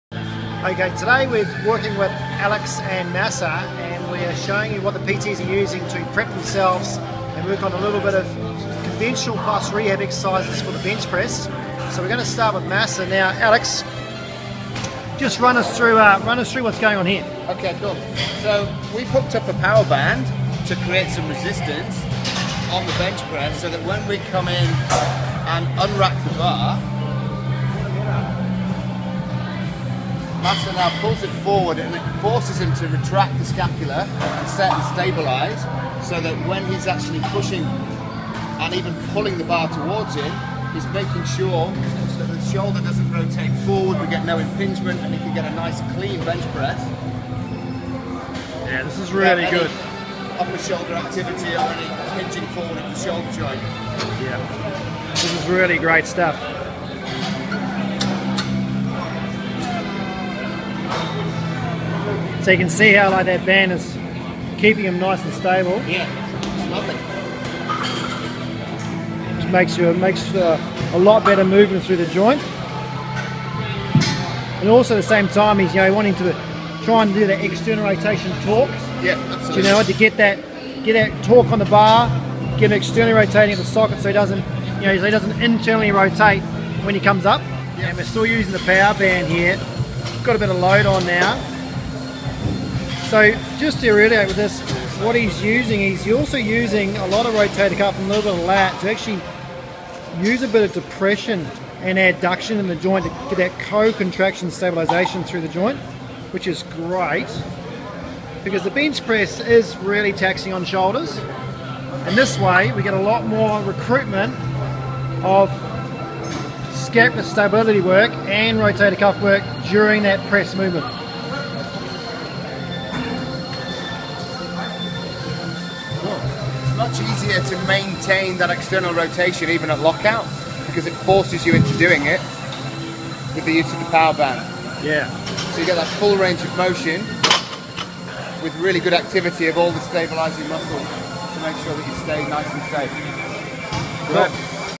Powerband to stabilise the bench press movement | Feat. Tim Keeley | No.48 | Physio REHAB
Personal Trainers Alex Hinchcliffe and Masa Yamaguchi demonstrate how to use a powerband to stabilise the scapula and maintain external rotation in the shoulder joint during a bench press